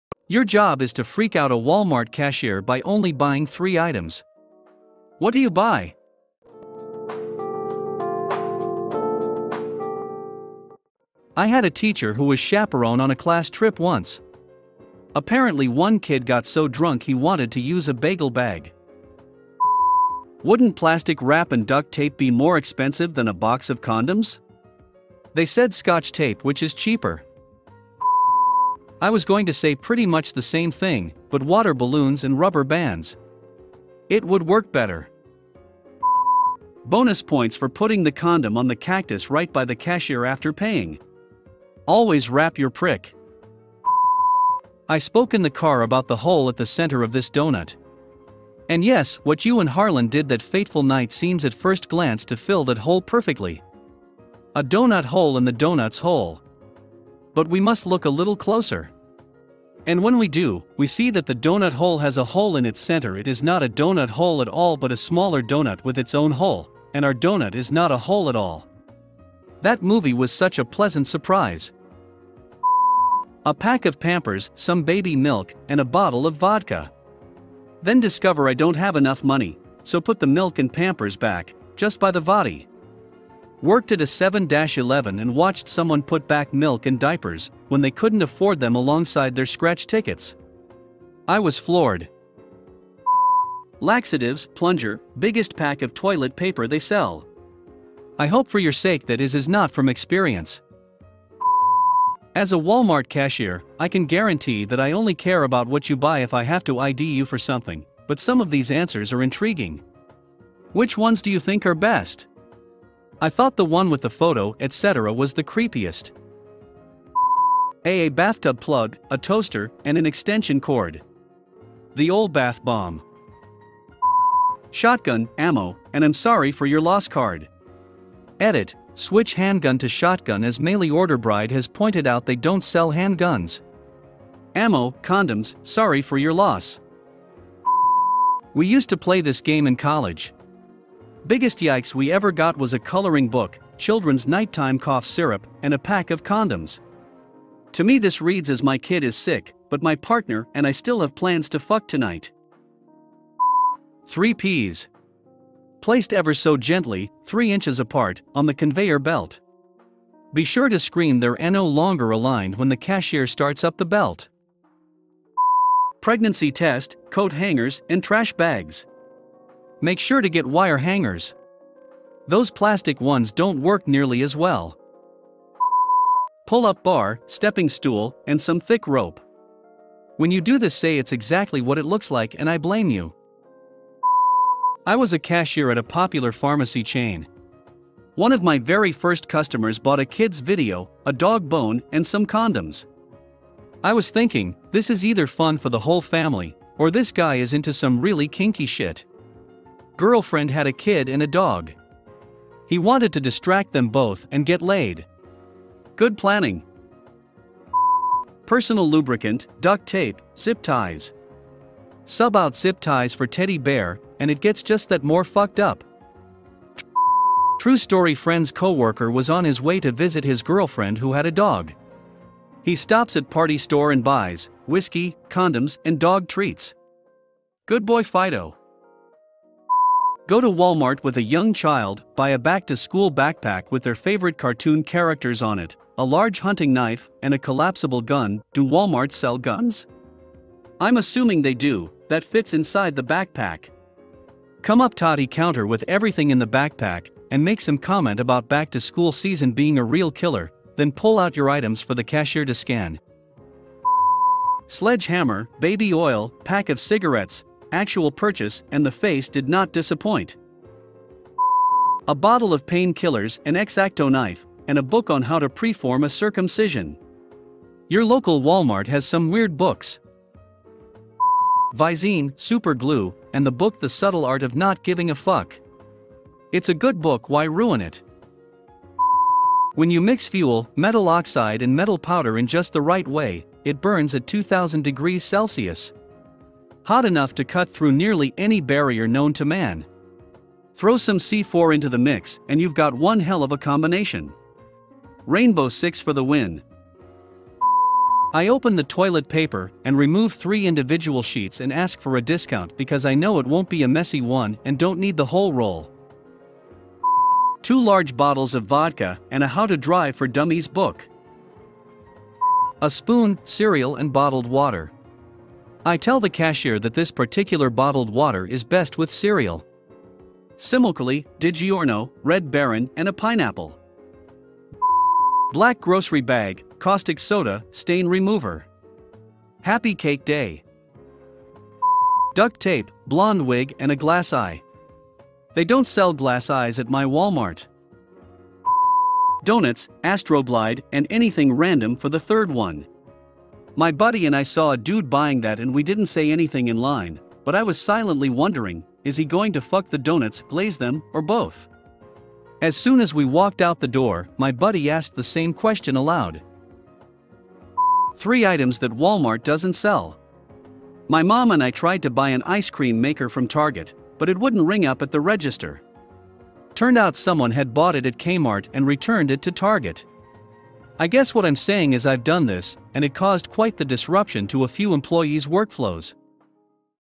Your job is to freak out a Walmart cashier by only buying three items. What do you buy?
Nothing as it doesn’t freak them out. I’ve had people buy lube, a child leash backpack combo, two LEGO Jurassic Park sets, slime, kids underwear, a bra so big it should be a tarp, a box set of Seinfeld seasons, an air fry popcorn maker, and a bottle of bleach.

The only reason I remember this is because they said “it’s not what it looks like”. Want to creep them out? Make up some fun stories. This lube isn’t for me. It’s for my sister. And she’s in a hurry so could you ring this up quickly.